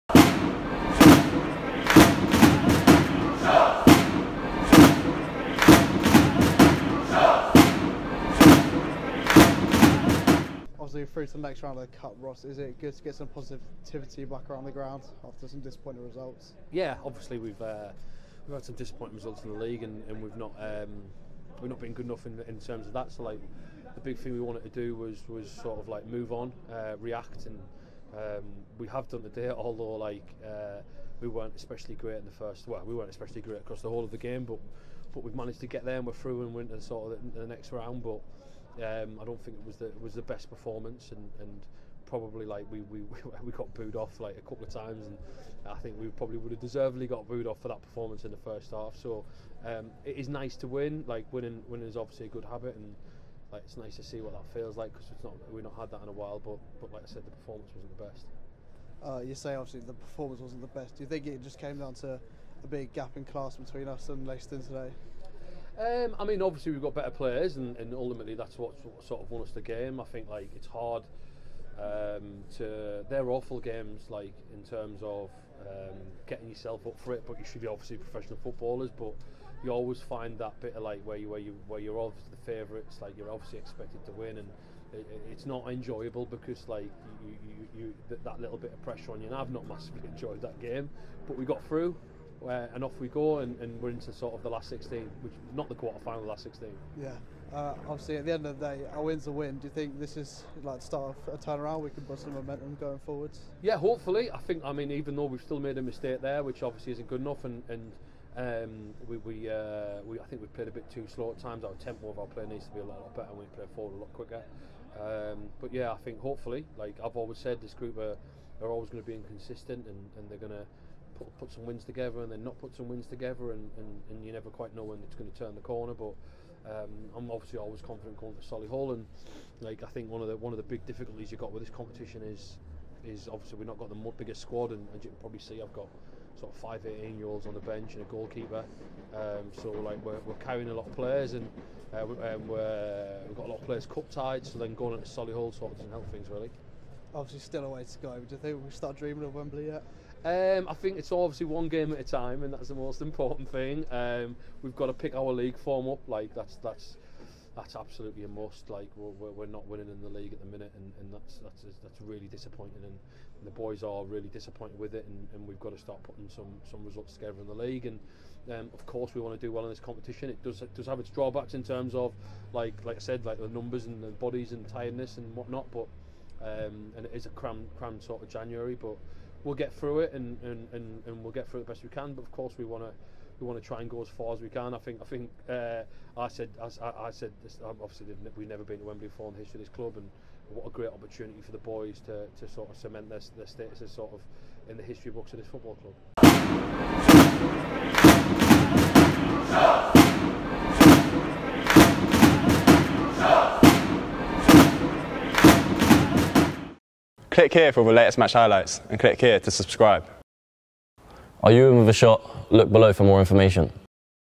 ROSS MCNEILLY POST-MATCH: Shots vs Leiston
Ross McNeilly gives his thoughts as The Shots progress in the Isuzu FA Trophy!